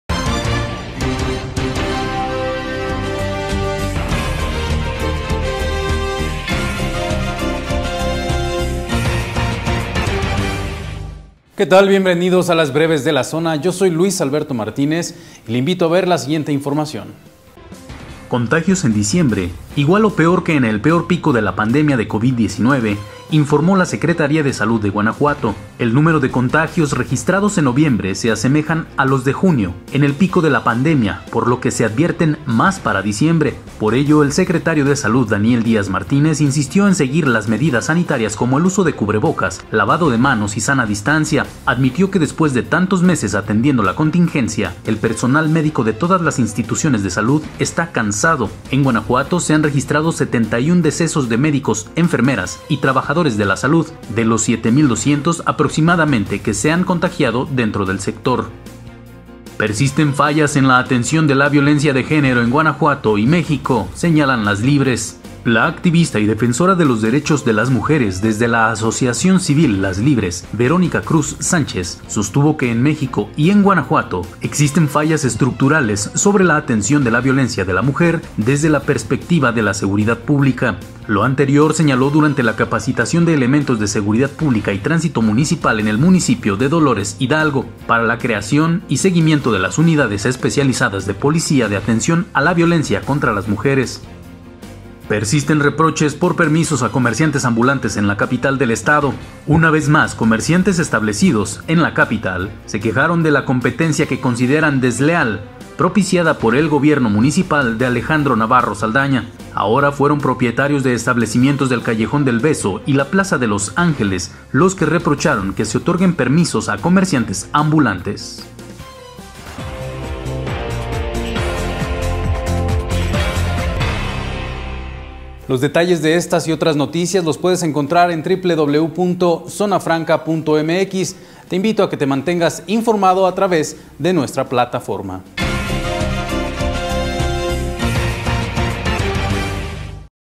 Contagios en diciembre igual o peor que en el peor pico de la pandemia | Breves de la Zona
- Insisten reproches por permisos a comerciantes ambulantes en la capital.
- Contagios en diciembre igual o peor que en el peor pico de la pandemia de COVID-19: SSG.
- Persisten fallas en la atención de la violencia de género en Guanajuato y México: Las Libres.